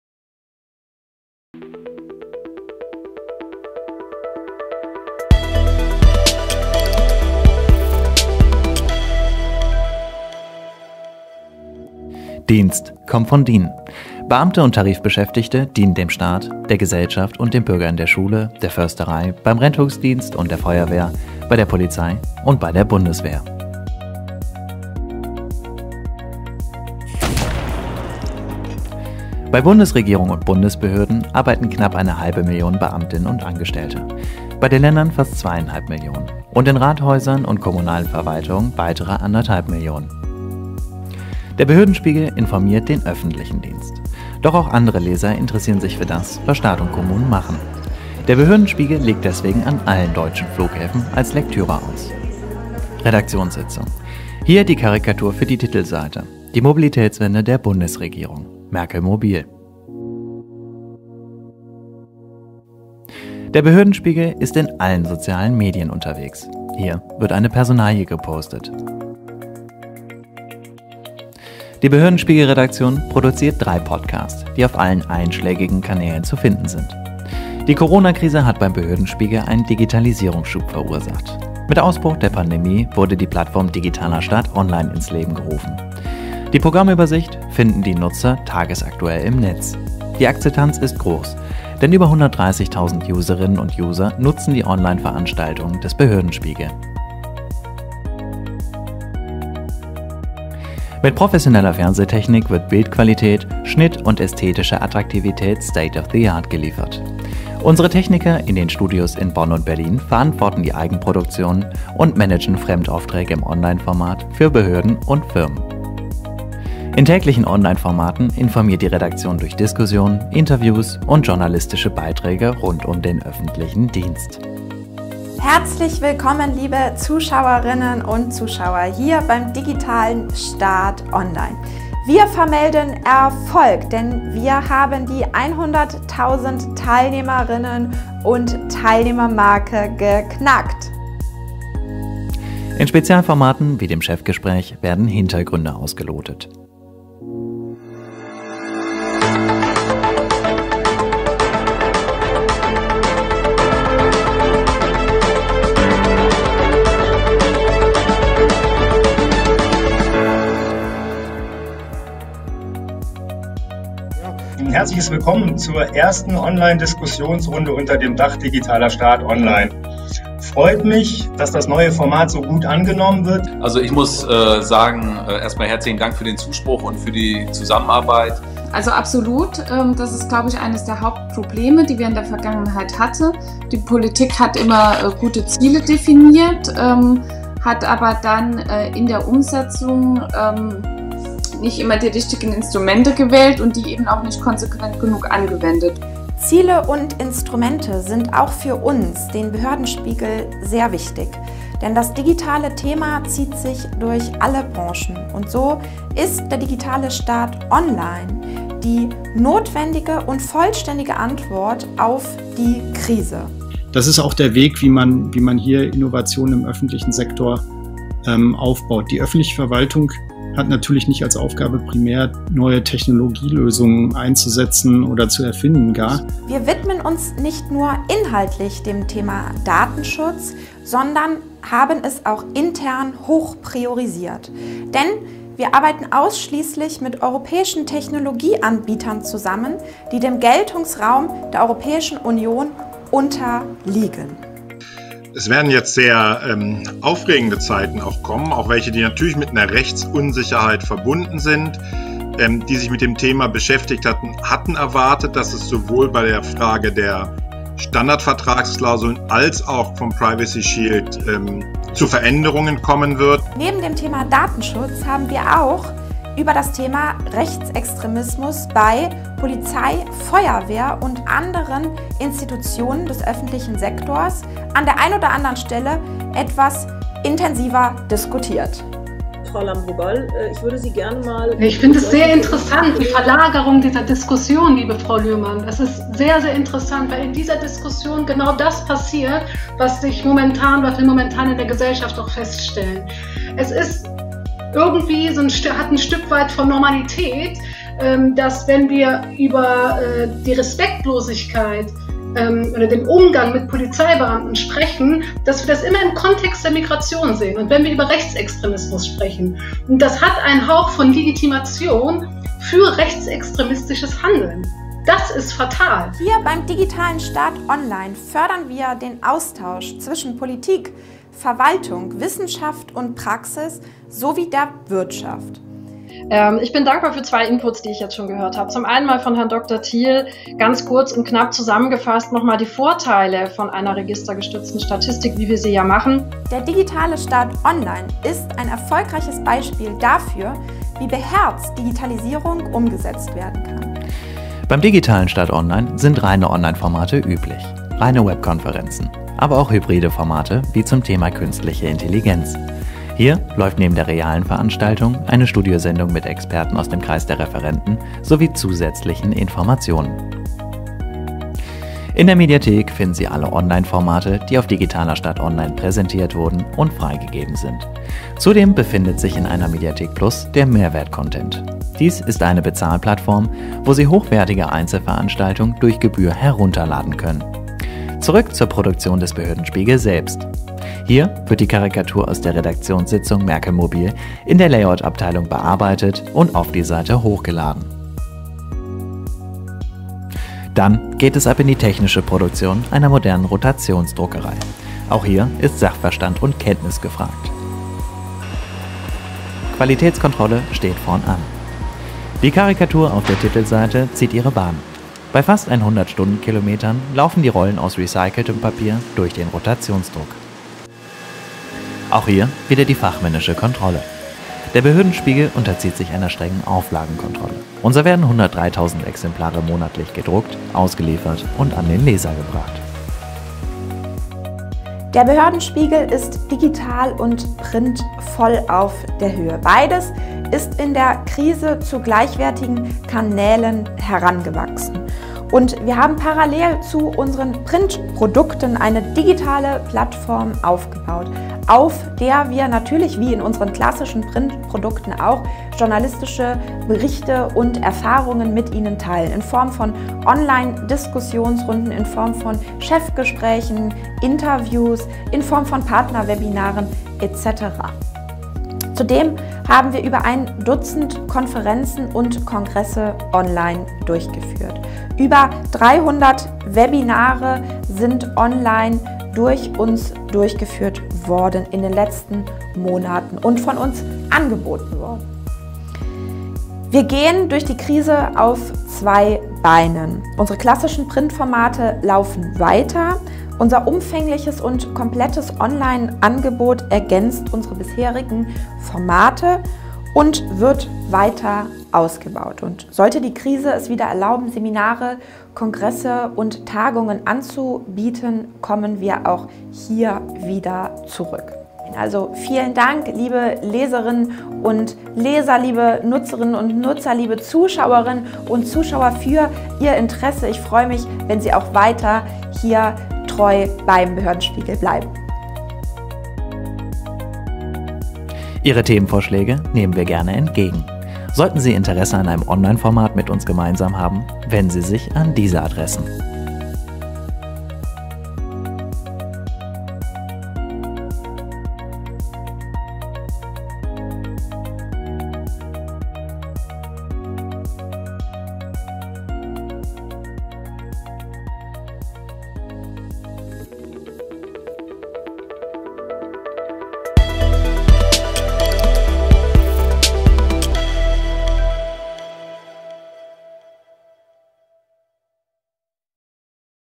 Leitmedium für den Öffentlichen Dienst – Behörden Spiegel inside
Der Behörden Spiegel präsentiert sich: Von der Produktion der Zeitung über die zahlreichen Online-Veranstaltungen, hybriden oder Web-Kongresse bis hin zu weiteren Projekten liefert unser Portrait Hintergründe und überraschende Perspektiven.
Wer oder was ist die Zeitung? Und was macht ein Verlagshaus im digitalen Zeitalter?

Erleben Sie hautnah, wie der Behörden Spiegel in der Druckerei im Rotationsdruck mit atemberaubender Geschwindigkeit produziert wird. Die User schauen der Online-Redaktion über die Schulter und nehmen im Studio des Digitalen Staats Online in der ersten Reihe Platz.